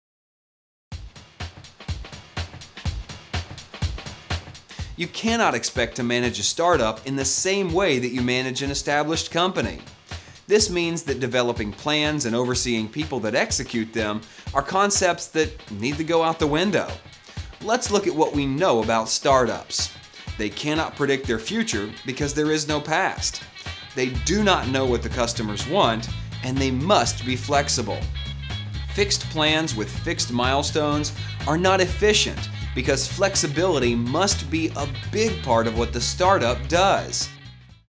LEAN STARTUP. PREVIEW by Coursmos
Eric Ries Lean Startup gives you the insight into how to shape a viable model for your business project. Here is a brief overview of his method’s core concepts.